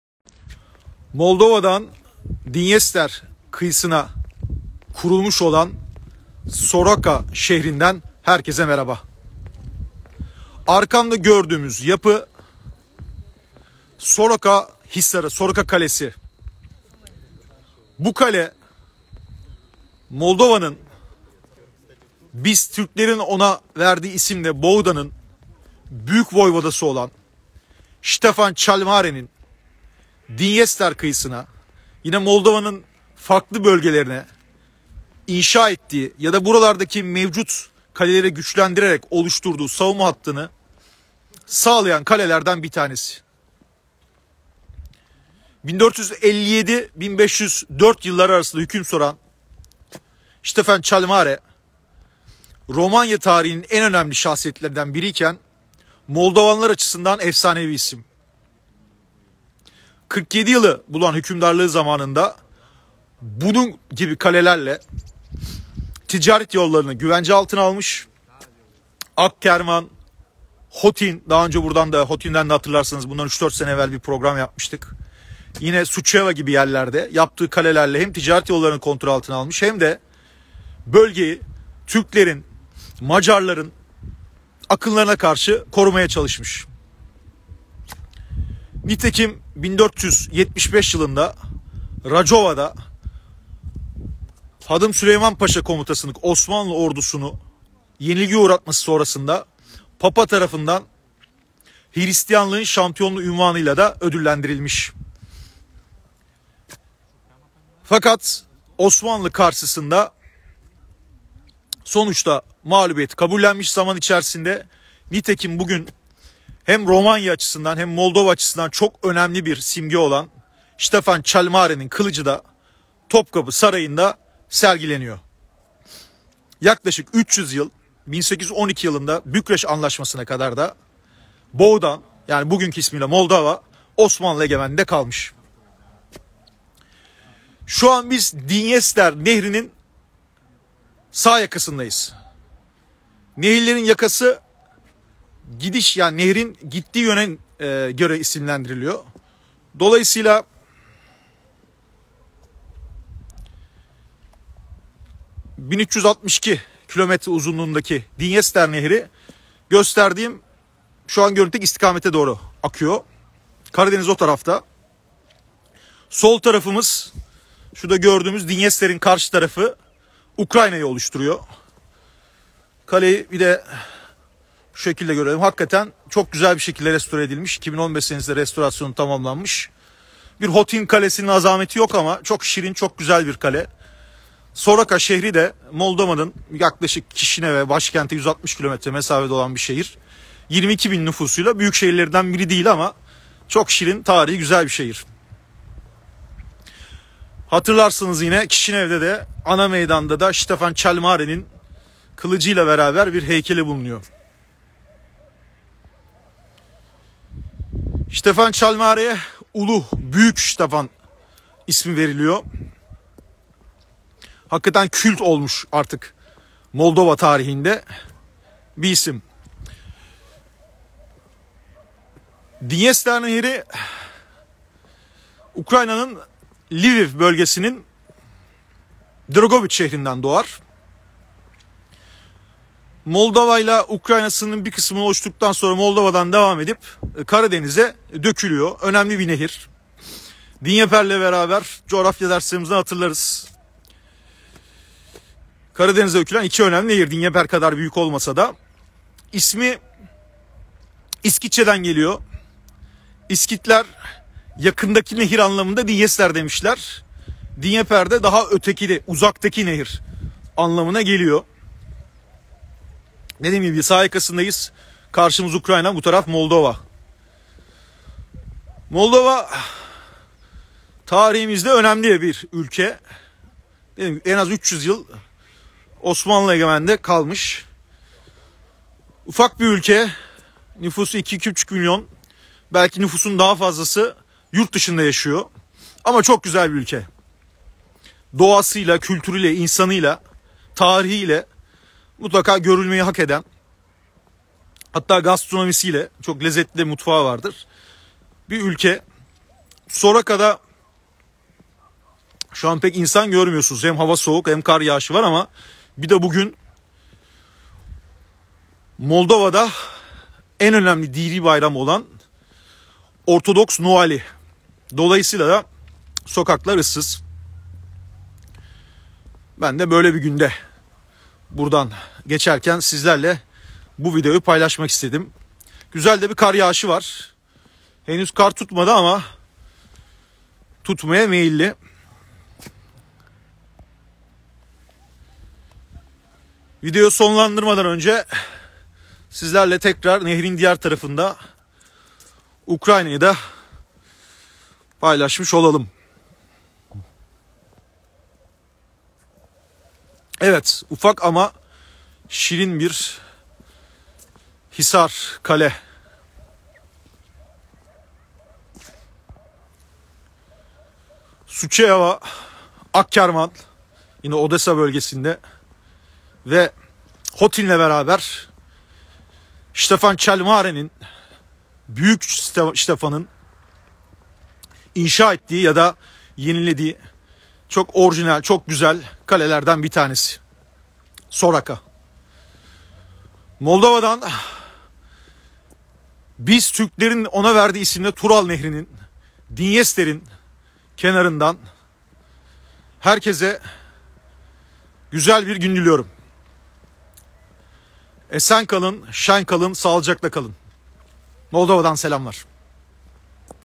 Moldova'nın dünü ve bugünü, Dinyester kıyısındaki Soroka Hisarından
Moldova’nın en güzel şehirlerinden Soroka’nın, Dinyester Nehri kıyısındaki Ukrayna’yı karşıdan gören aynı isimli kalesinin, Moldova, biz ona Türkler’in verdiği isimle Boğdan’ın tarihi açısından olduğu kadar, bizim tarihimiz açısından da önemi büyük. 300 yıl aynı devletin parçası olduğumuz Moldova'nın tarihi ve bugünü, şehri süsleyip güzelleştiren Dinyester'in, Türkçe ismiyle Turla'nın kenarından, Soroka Hisarı'nın önünden